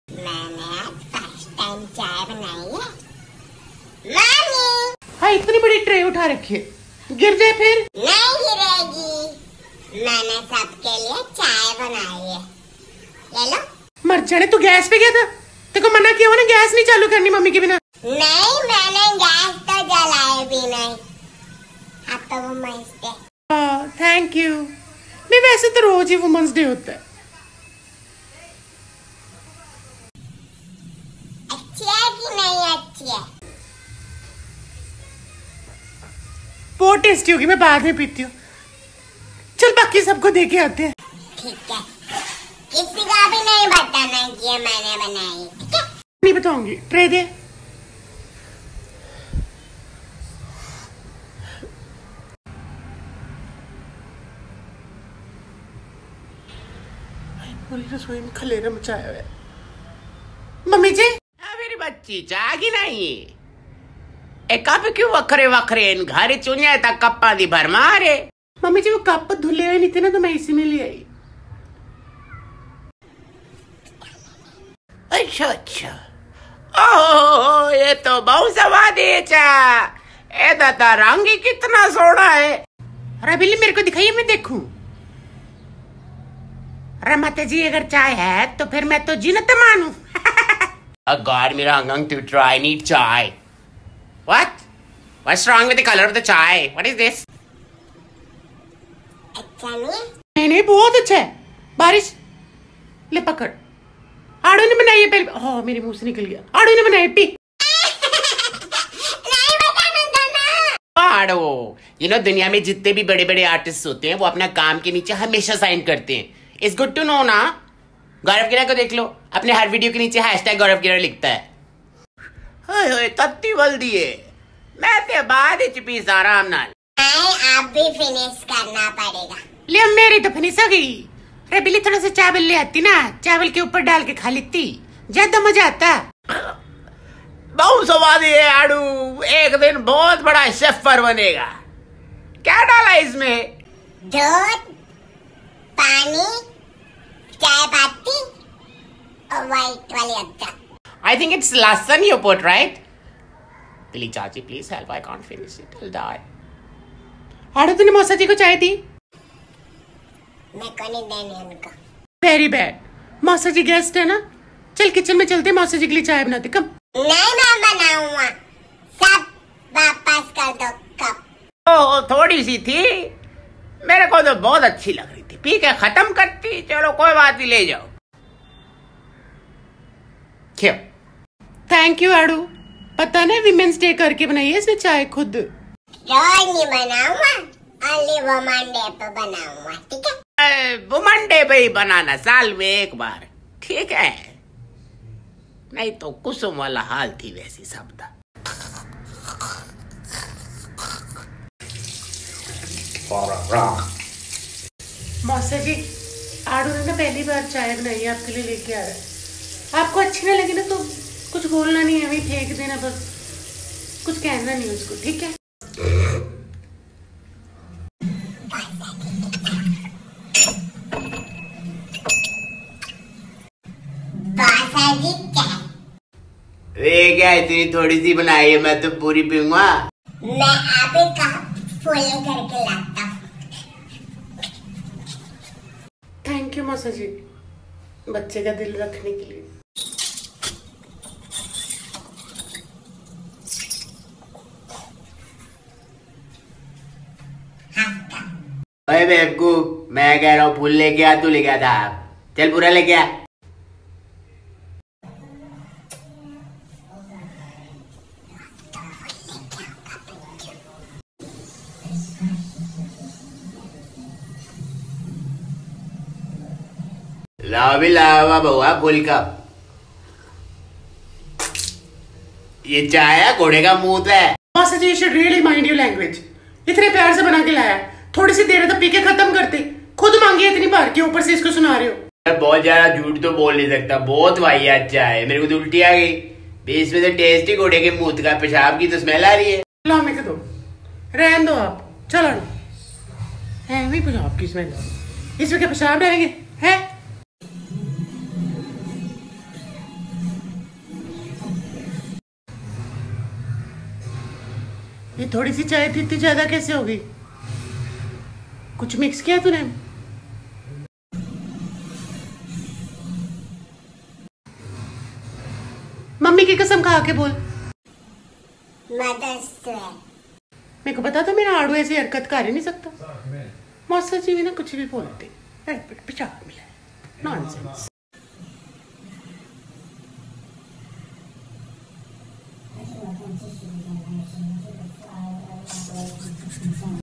Aadu Ne Womens Day Pe Chai Banai | Gaurav Gera
Aadu makes tea for all the family members and then mausa ji asks for tea